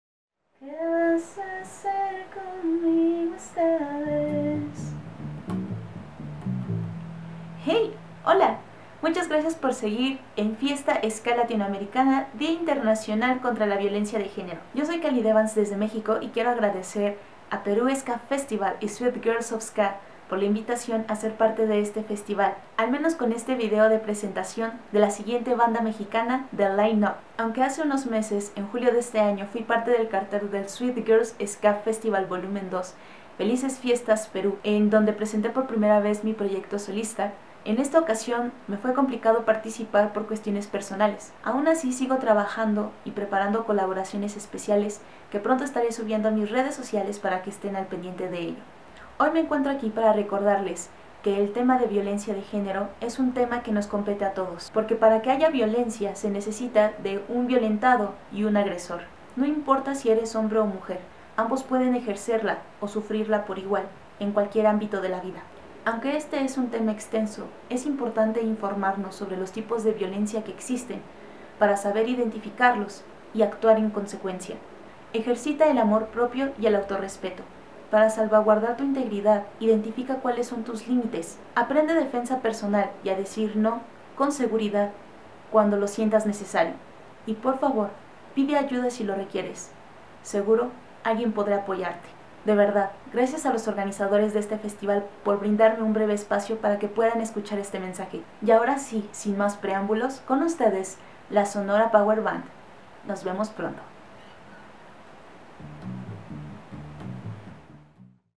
Kalid Evans presenta a La Sonora Power Band Fiesta Ska Latinoamericana
Gracias @kalid.evans, una gran profesional del mundo SkaMexicano que estuvo presente en SweetGirlsSkaFestivalInternacional FelicesFiestasPeru en Julio, en esta oportunidad en FiestaSkaLatinoamericana DiaInternacionalContralaViolenciadeGenero presentando a la banda @lasonorapowerband